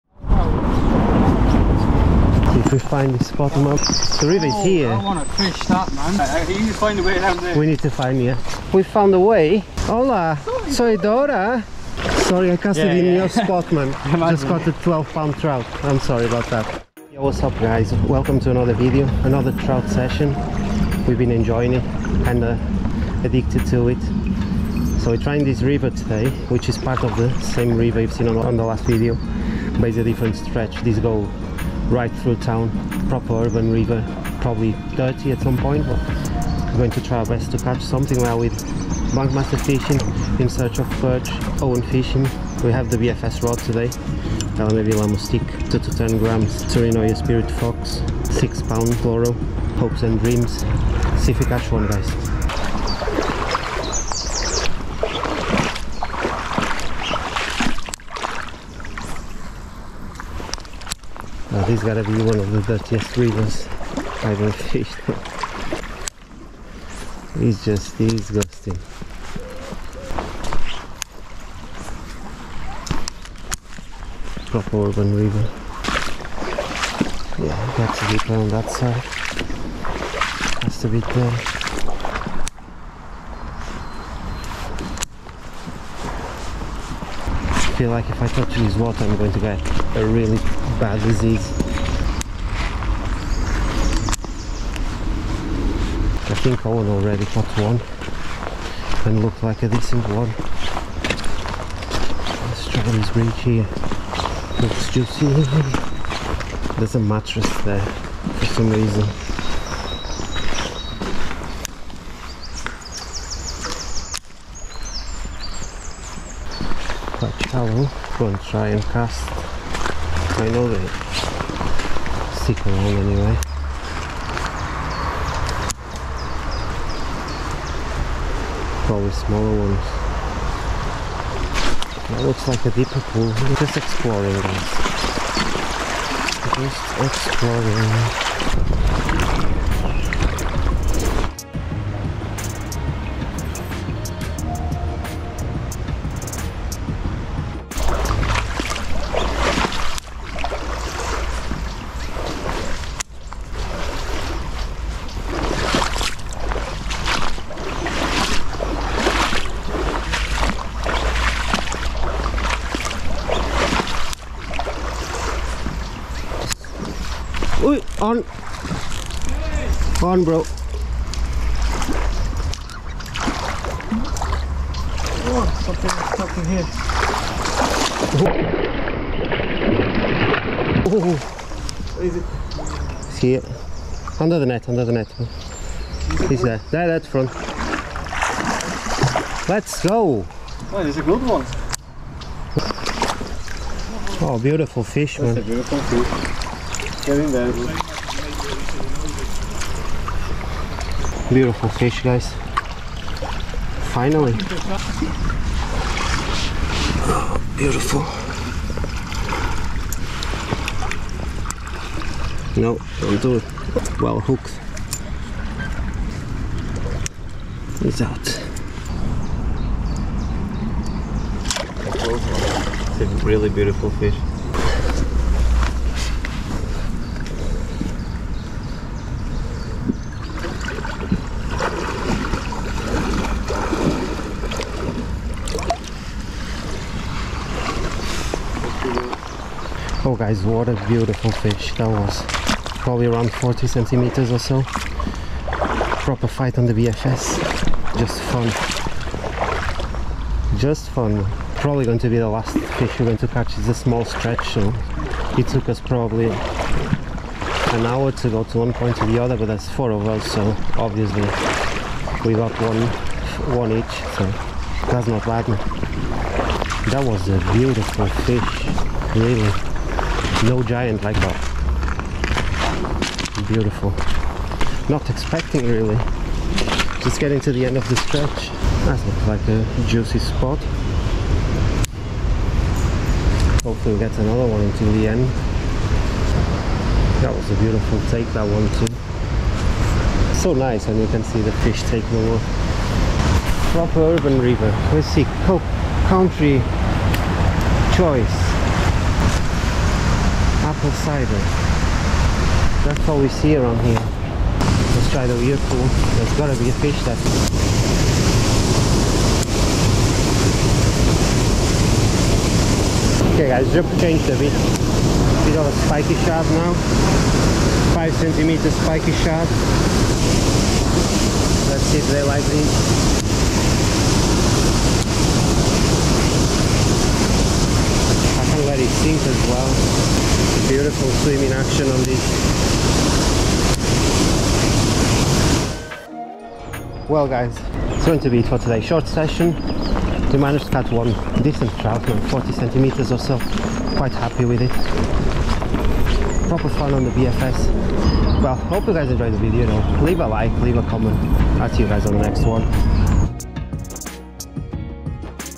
Urban BFS Fishing for Trout Manchester City Centre (Stunning Fish)
Short session fishing one of the urban rivers that go right through city centre! Was dirty, smelly but had a little gem living on it! Probably the prettiest wild brown trout I've ever caught!

- Gear

LMAB La Moustique casting rod 2-10g

Tsurinoya Spirit Fox BFS Reel

Tight lines,

Carlos
ZombieBiteFishing